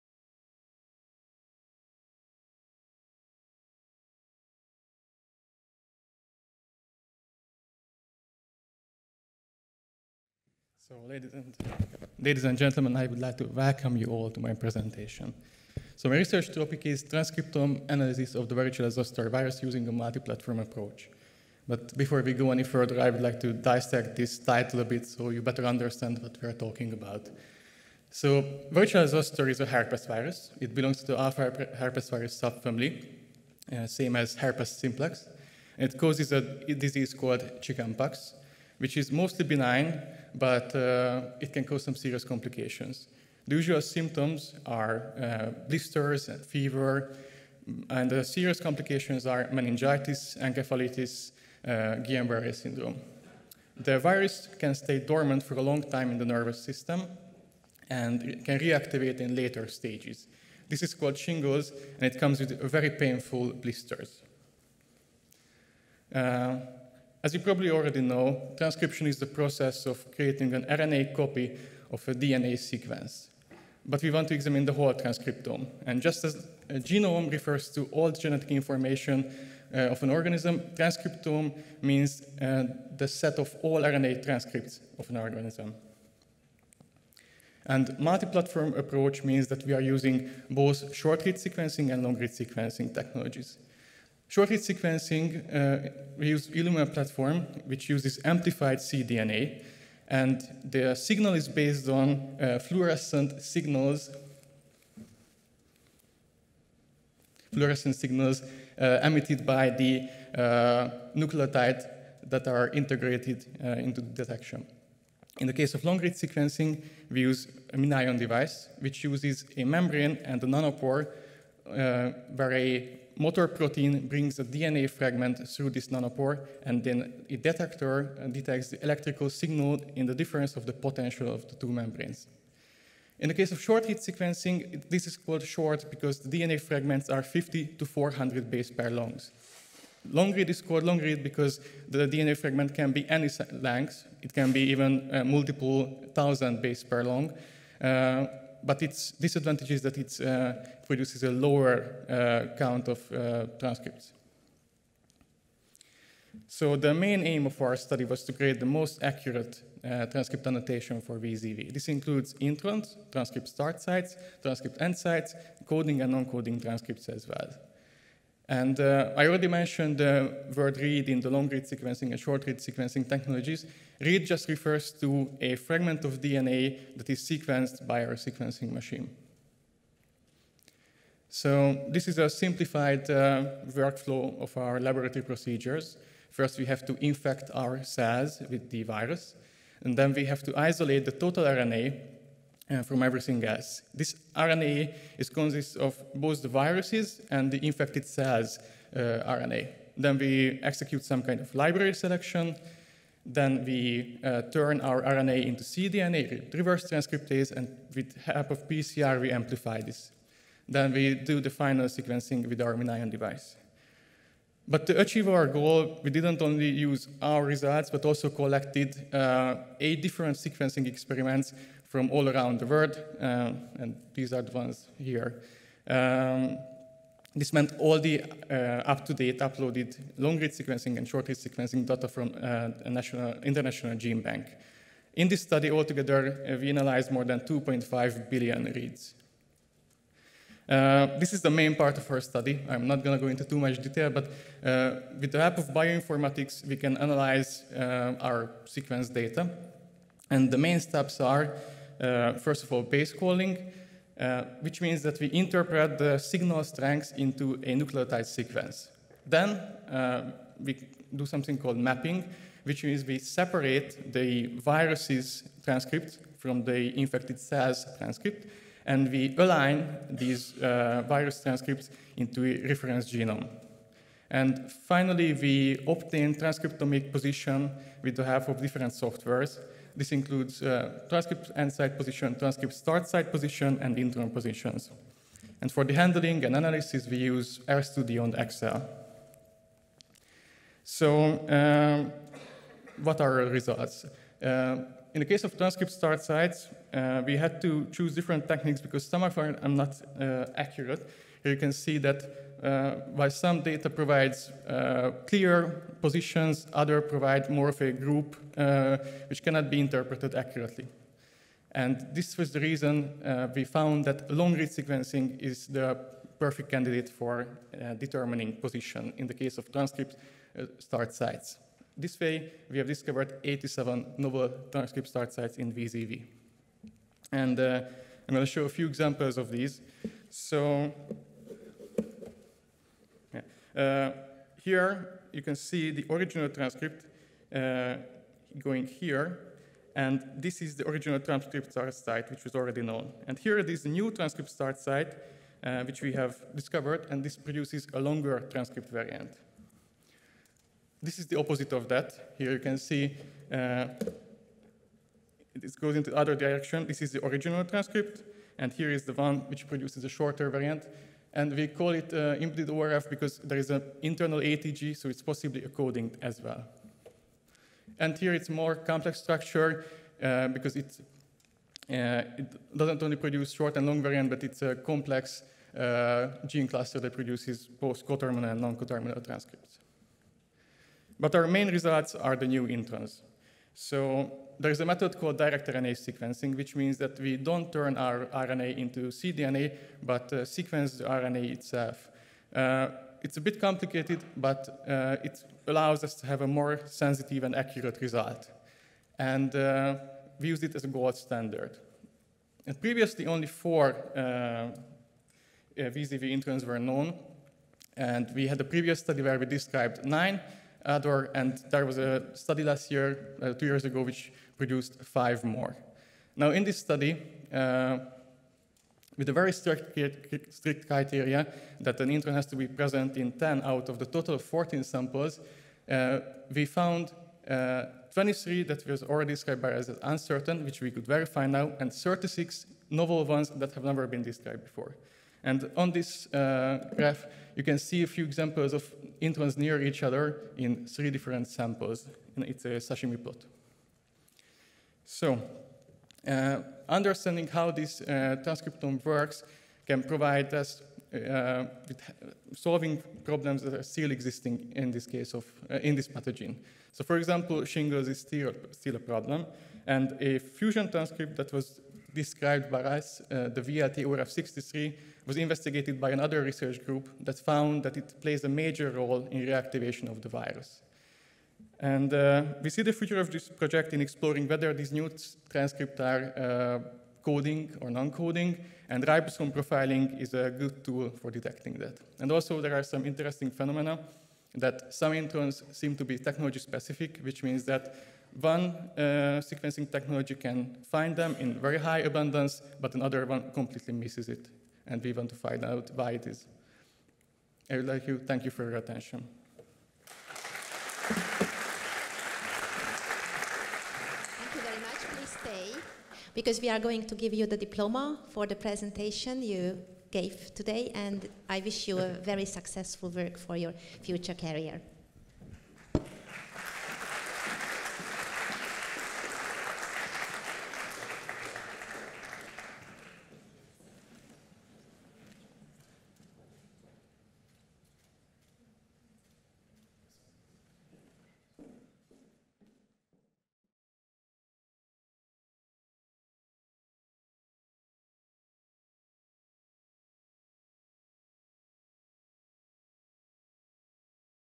Harangozó Ákos előadása (Ákos Harangozó's lecture)
Nobel-díjasok és tehetséges diákok XIX. találkozója
Szeged, 2022. november 10-12.
Vendég: Roska Botond
19th Meeting of Nobel Laureates and Talented Students
Szeged, November 10-12, 2022
Guest: Botond Roska